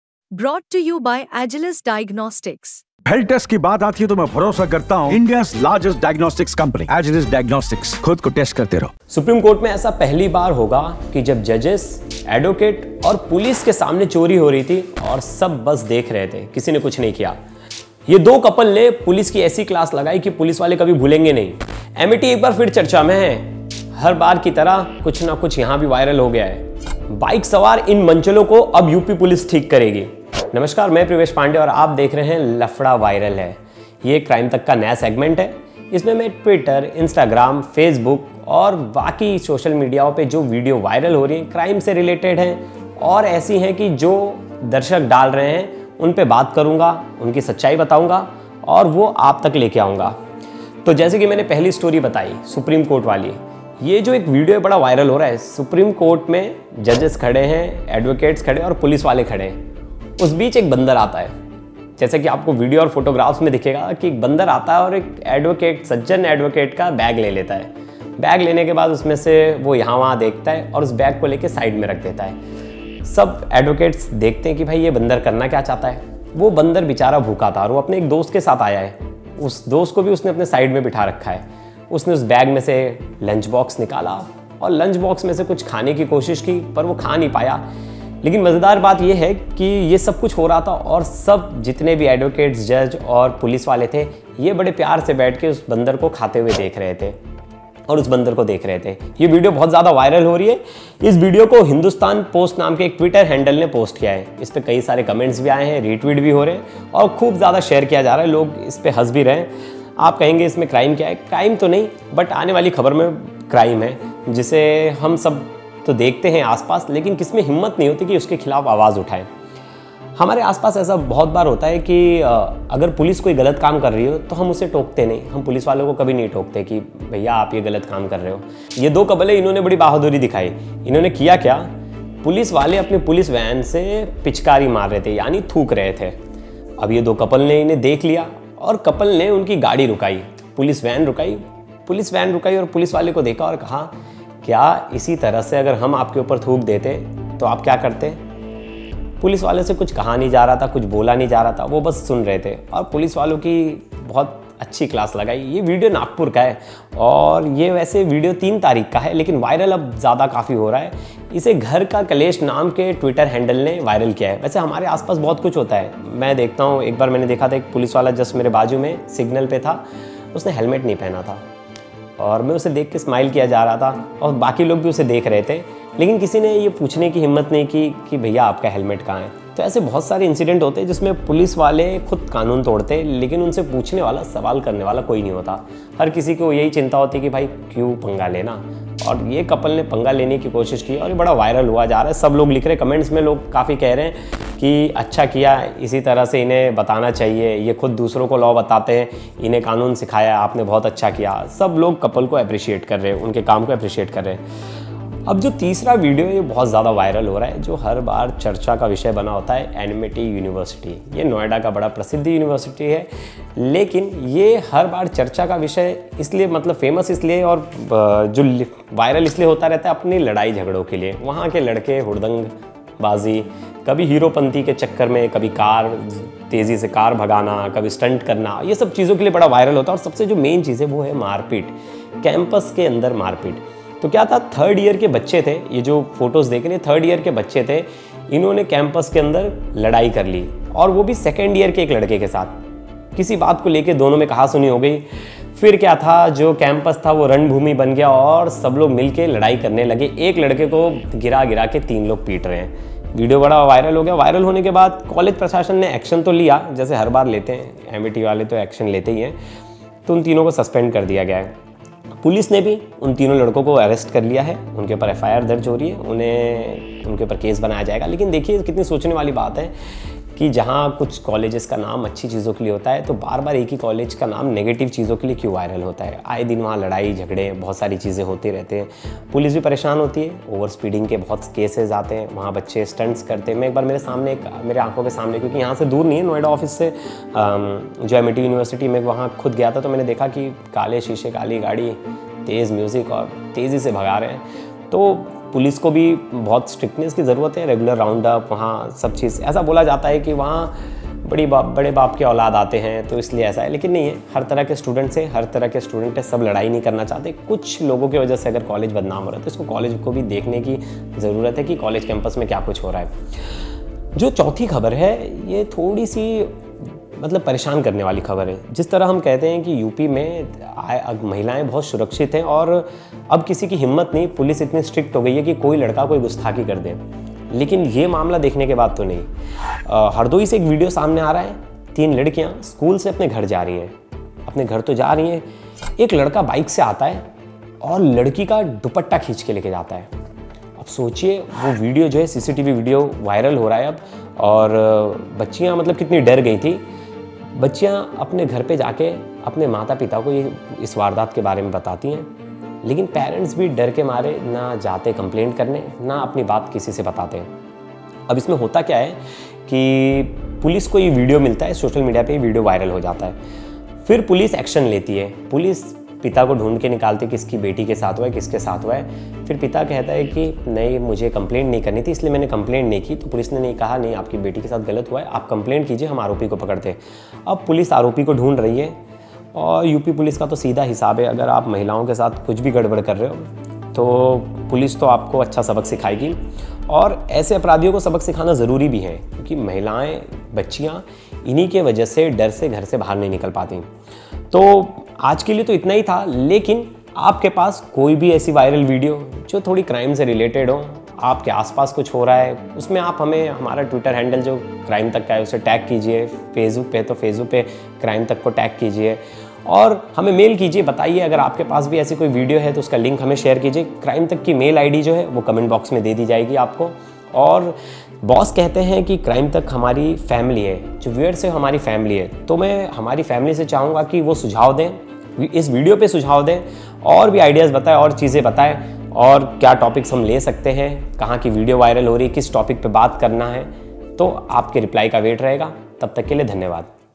लफड़ा VIRAL है..... इस कपल ने दरोगा की जो क्लास लगाई वो Police कभी नहीं भूलेगी
Privesh Pandey Report